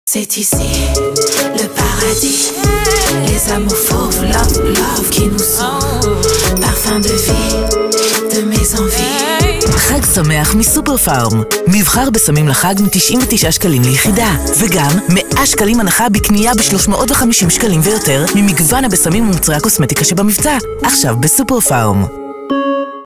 חג שמח מסופר-פארם
מבחר בשמים לחג מ-99 ₪ ליח' וגם 100 ₪ הנחה בקנייה ב-350 ₪ ויותר ממגוון הבשמים ומוצרי הקוסמטיקה שבמבצע.